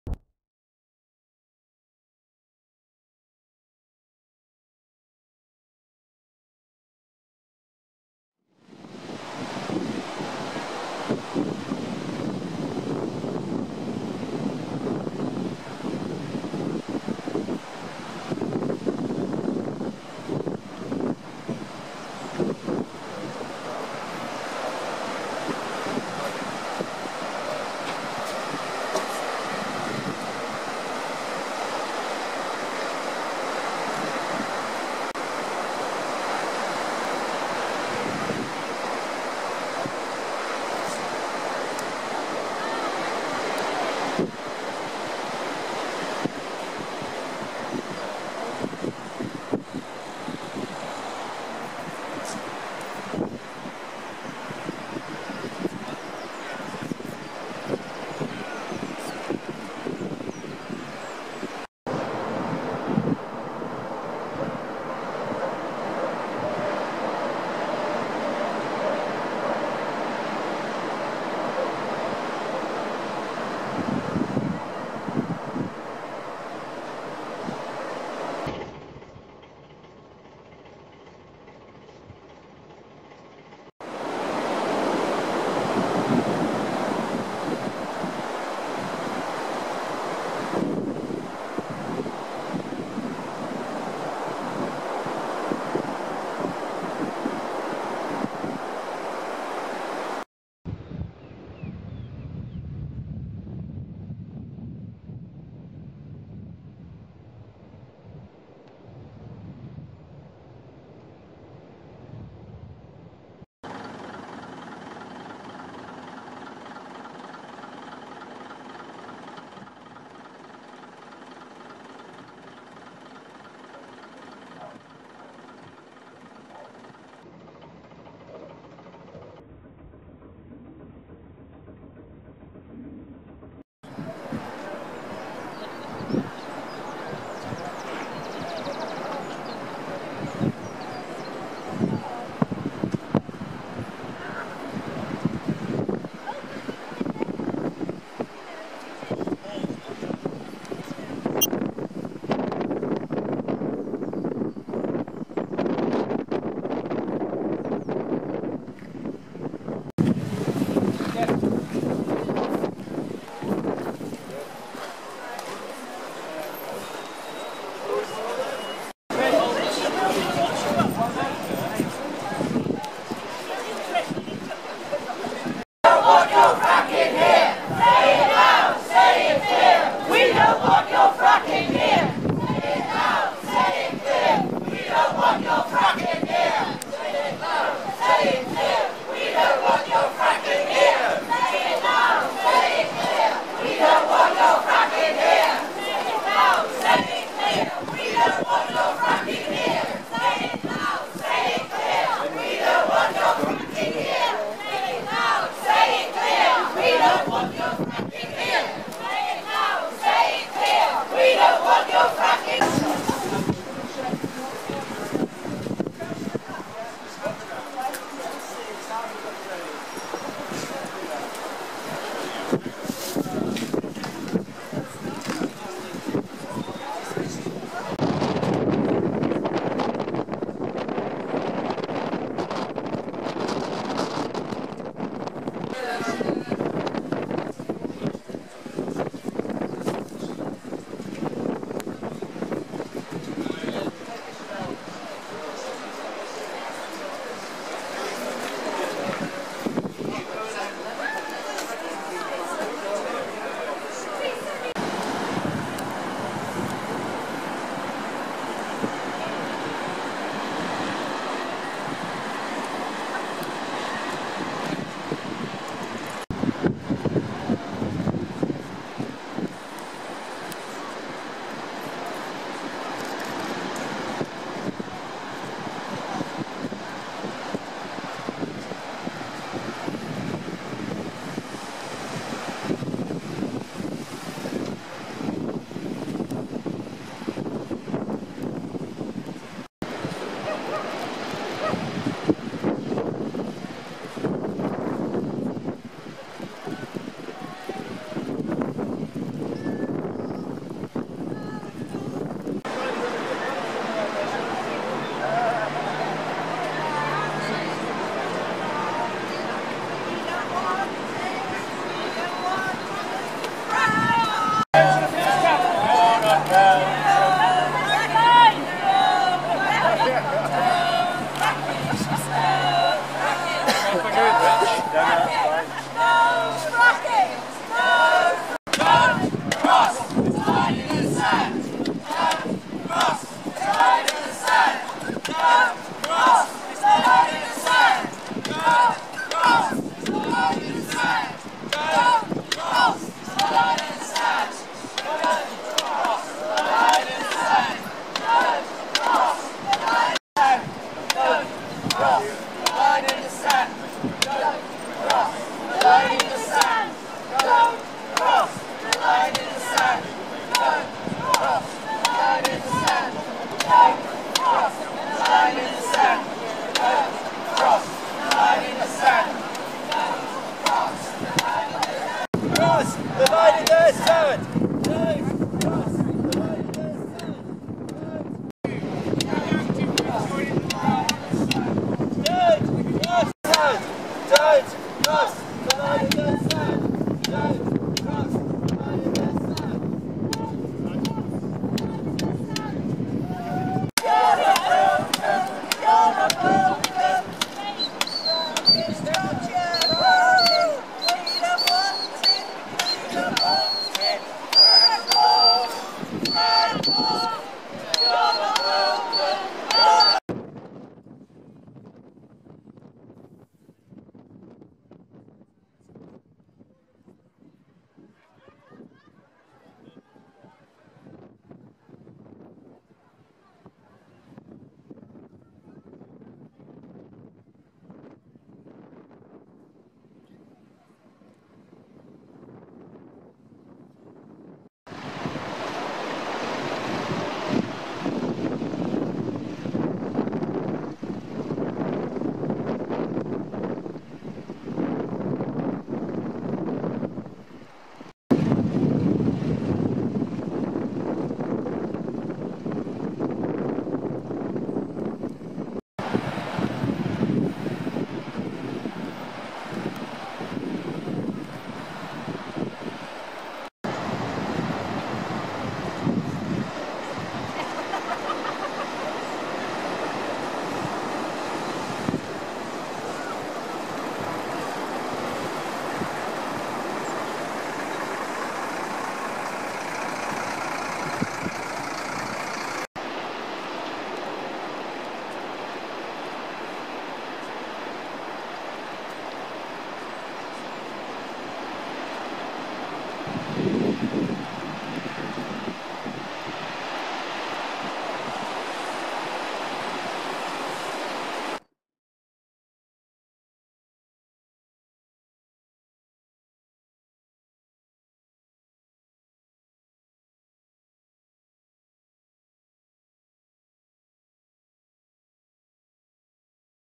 ANTI FRACKING DEMO SCARBOROUGH
Video of Anti-Fracking Demo held at the Spa, Scarborough in response to the undemocratic Pro-fracking conference being held at the Spa conference centre, Scarborough on 25th April 2016.